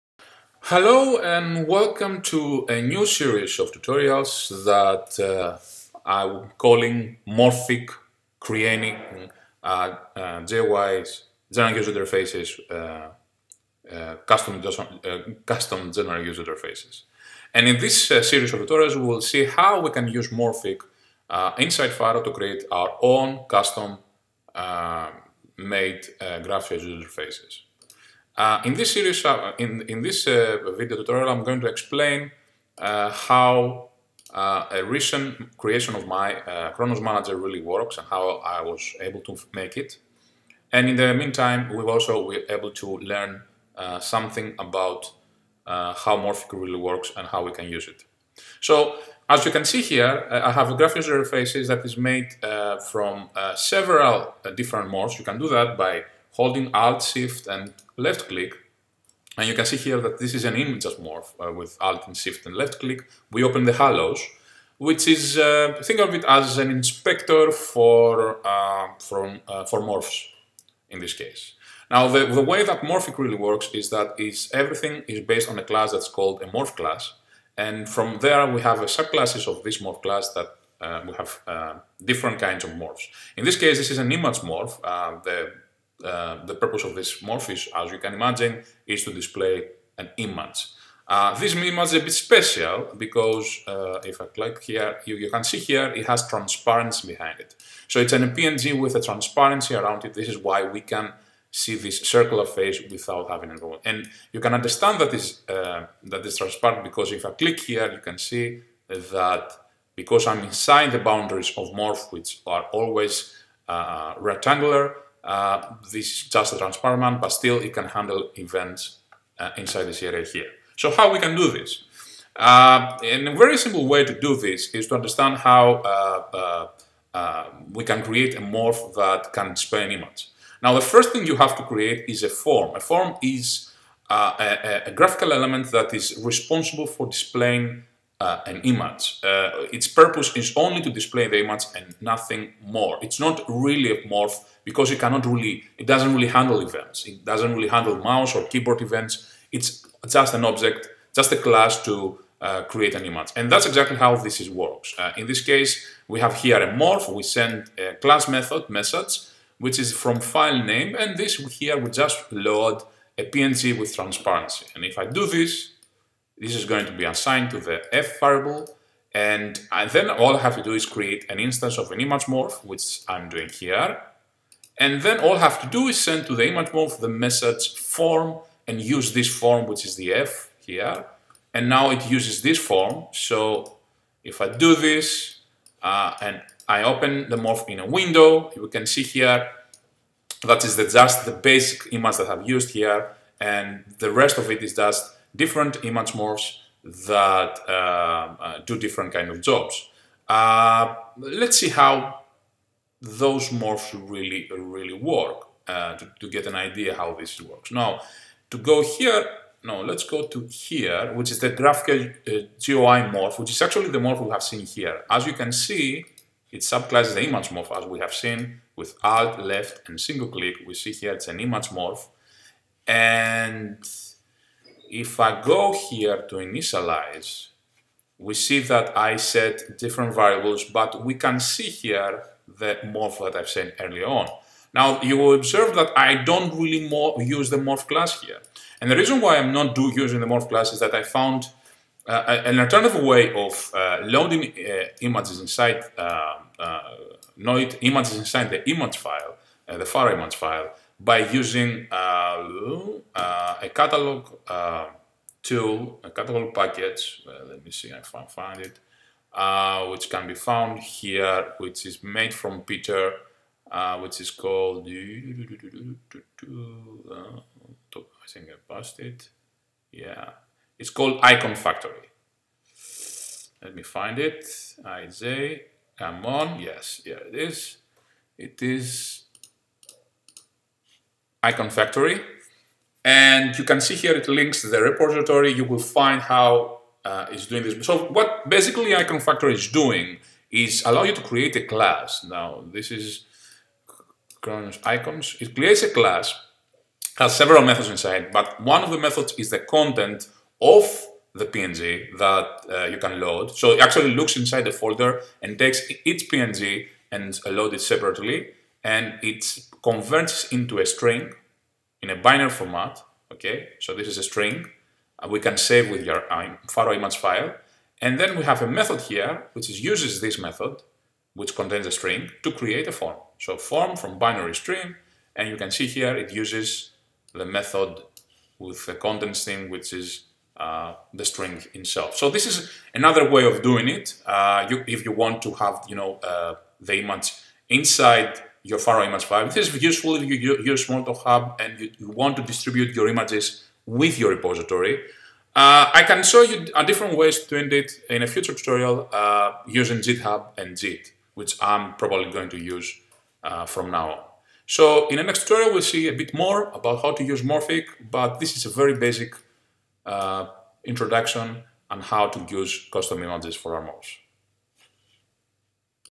[ Pharo - Morphic 001] Using a custom image for a morph
How to use a png to create a custom morph in pharo